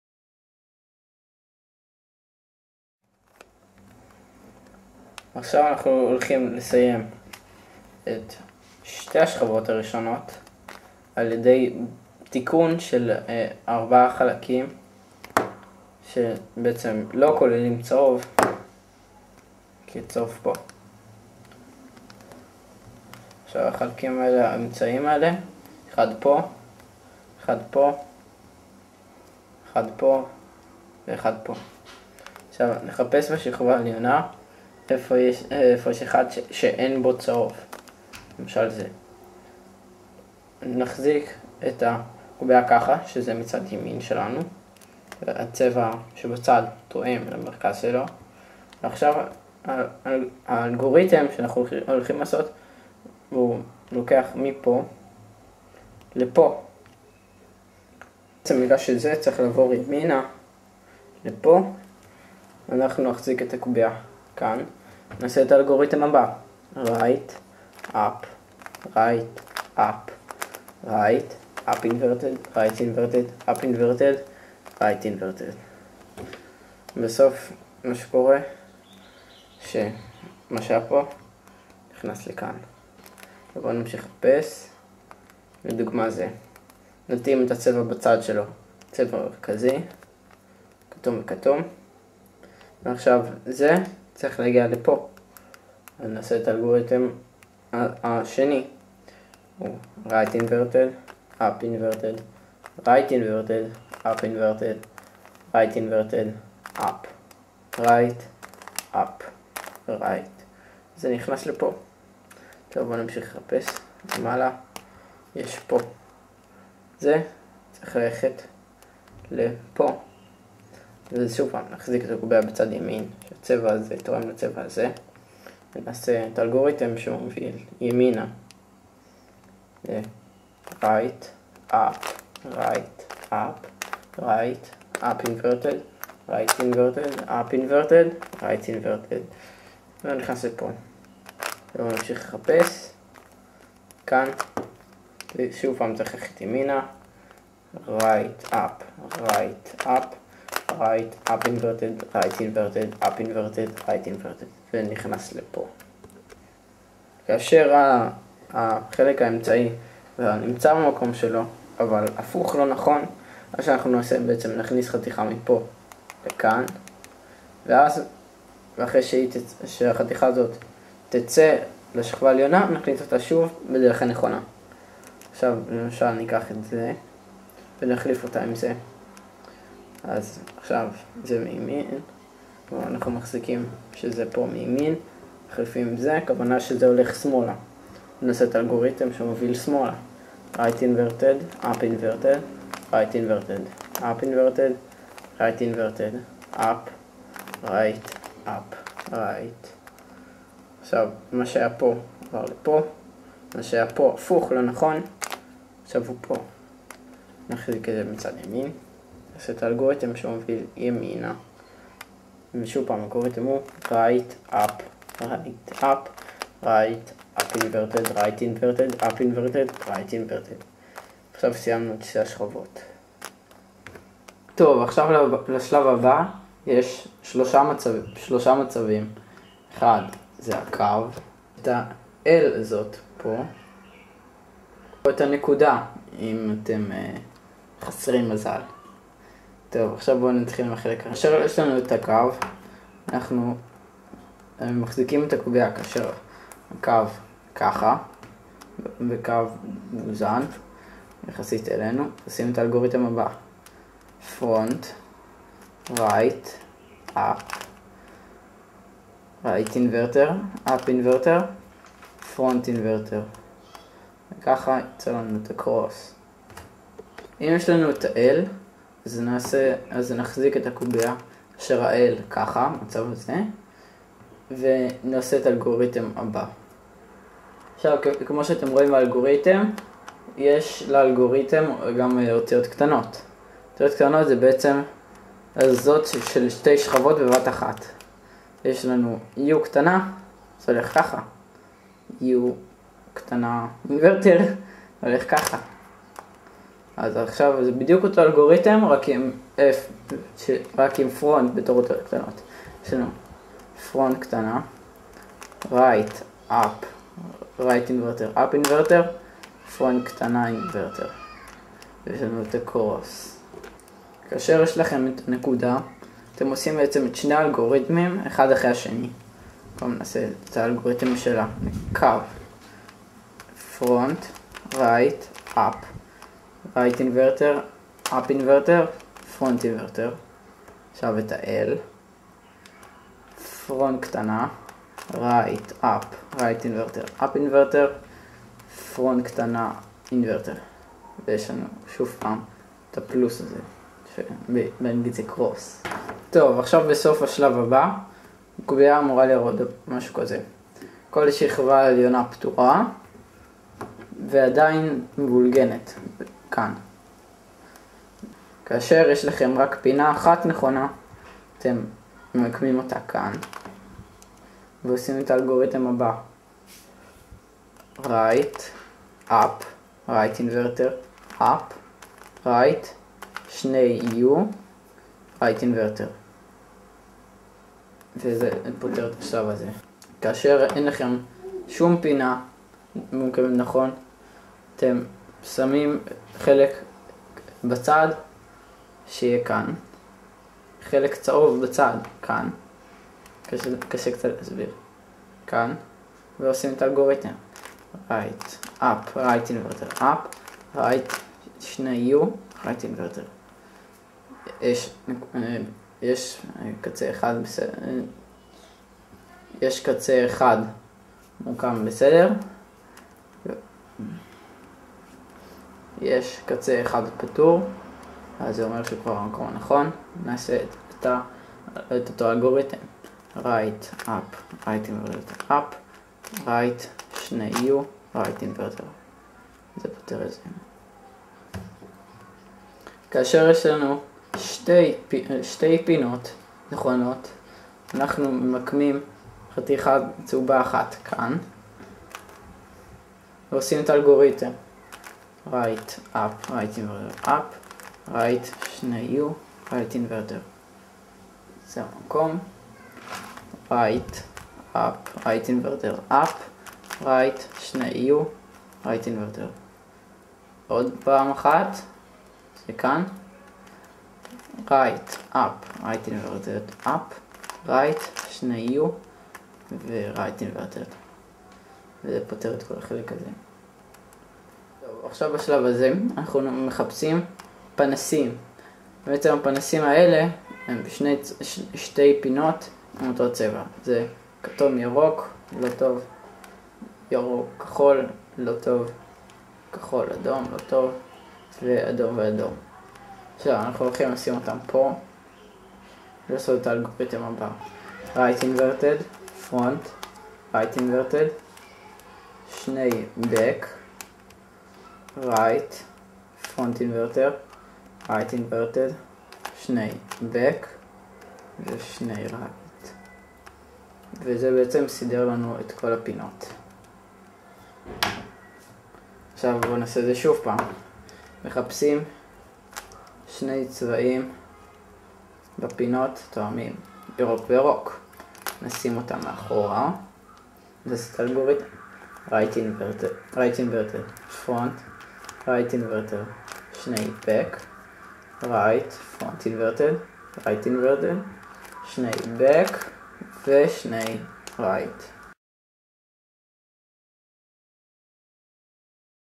מדריך לפתירת הקוביה ההונגרית חלק שני
פתירת הקוביה ההונגרית חלק שני
שתי השכבות הראשונות- להזיז חתיכה מלמעלה למטה מצד ימין:
R U R U RU' R' U' R'        1
להזיז חתיכה מלמעלה להזיז למטה לצד שמאל:(אין שום דרך אחרת להסביר)
R' U' R' U' R' U R U R
רוצים להשתתף באליפות הארץ בקוביה הונגרית? שלחו מייל ל:
בשלב הבא יש שלושה מצבים: קו ,L, ונקודה.
האלגוריתם של הקו-F R U R' U' F'       1
L(אל)- f R U R' U' f'                        1
נקודה- שני האלגוריתמים(קודם את הקו ואז את ה L.
בשלב הבא נסדר את השכבה העליונה(הצהובה) בעזרת האלגוריתם הבא:
R U R' U R U2 R'        1
בשלב הבא נפתור את החלקים הפינתיים ע"י האלגוריתם:
R' F R' B2 R F' R' B2 R2
ברגע שיהיו לי 10 subsribers אז אני אפרסם סרטונים קבוע אז
SUBSRIBE!